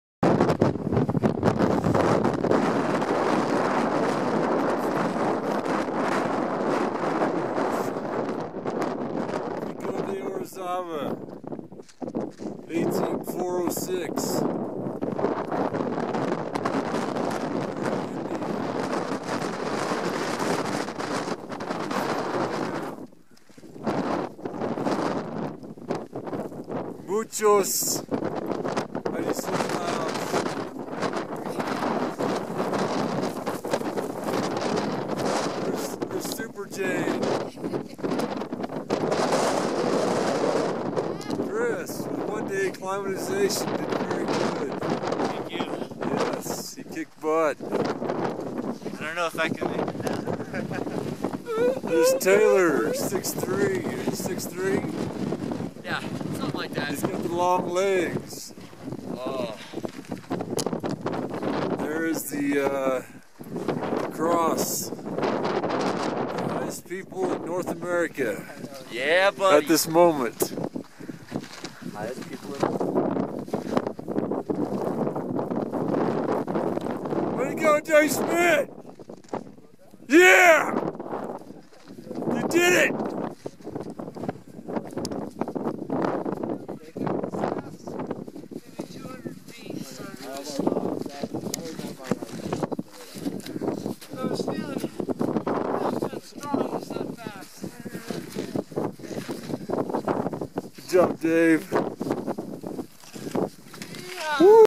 Orizaba summit views
Pico de Orizaba is the 3rd highest mountain in North America at 18406' and highest in Mexico.  Here are the views from the top a few days after a storm, taken 12/28/14.